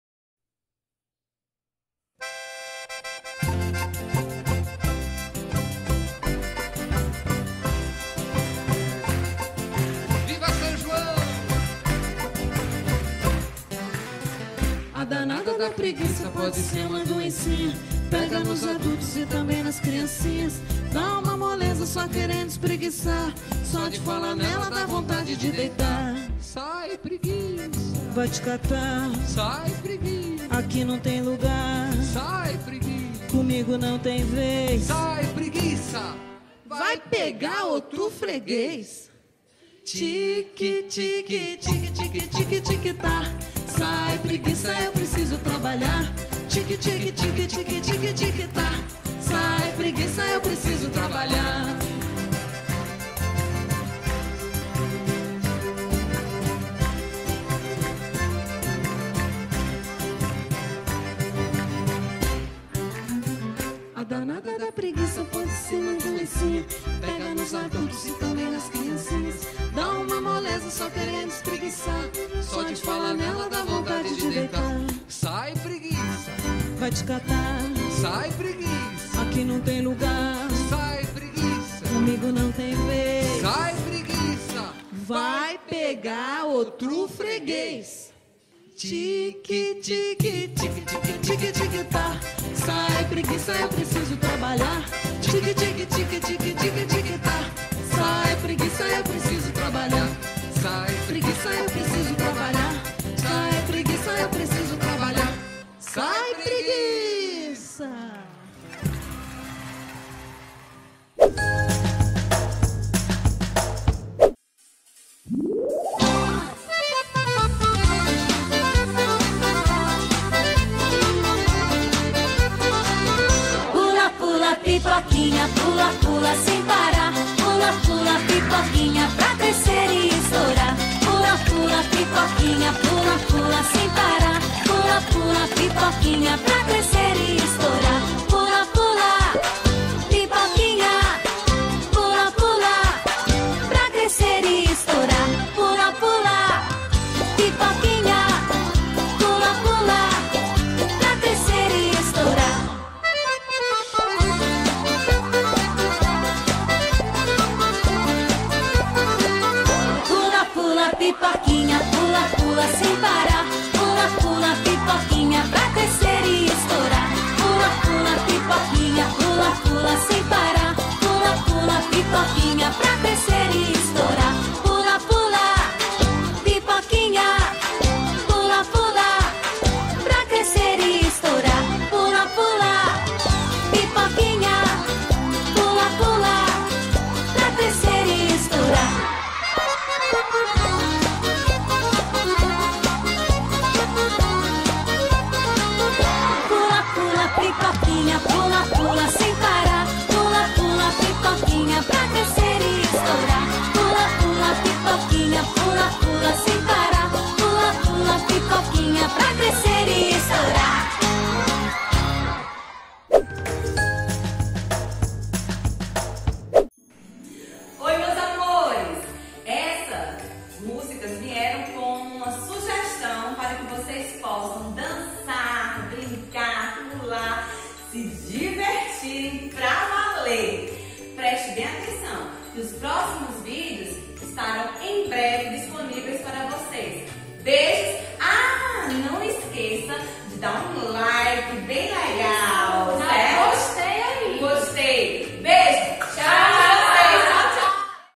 Interação 6
No vídeo de hoje, as professoras da Educação Infantil reuniram-se  para  possibilitar um momento de interação e diversão.

Professoras: Rita de Cássia, Tatiane Braga, Thalita Batista e Yana Moreno.

Filmagem: Paulo Cezar - PC.
Edição de vídeo: Matheus Fernandes.